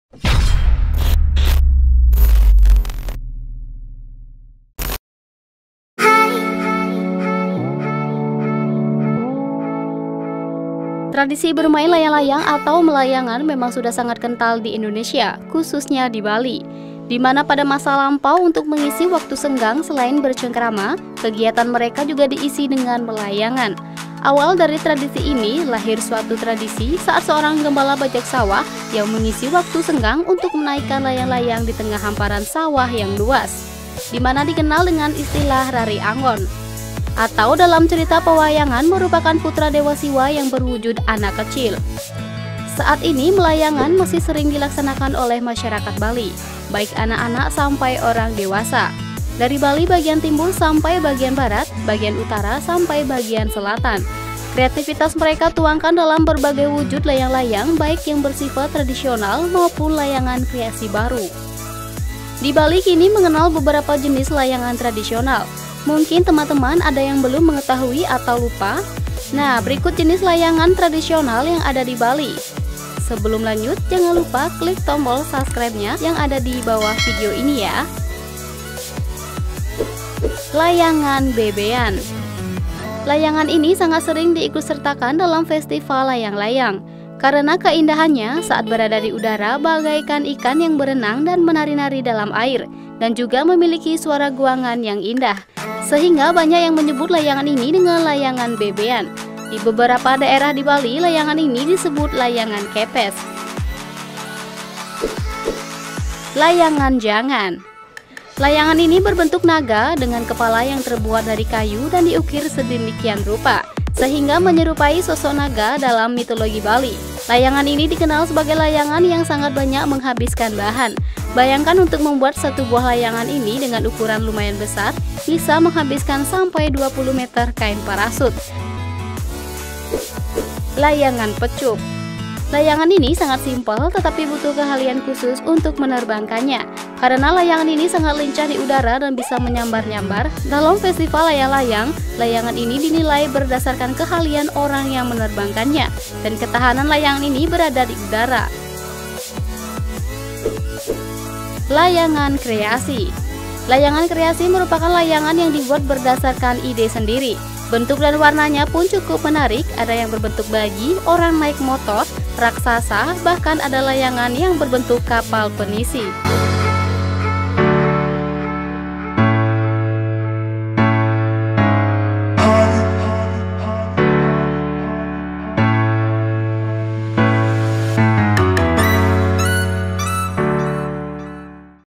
Sejarah dan Jenis-Jenis Layangan Tradisional di Bali
Tradisi bermain layang layang atau melayangan memang sudah sangat kental di indonesia khususnya di bali, dimana pada masa lampau untuk mengisi waktu senggang selain bercengkrama kegiatan mereka juga diisi dengan melayangan. Awal dari tradisi ini, lahir suatu tradisi saat seorang gembala bajak sawah yang mengisi waktu senggang untuk menaikkan layang-layang di tengah hamparan sawah yang luas. Dimana dikenal dengan istilah “Rare Angon” ( dalam cerita pewayangan merupakan putra dewa siwa yang berwujud anak kecil )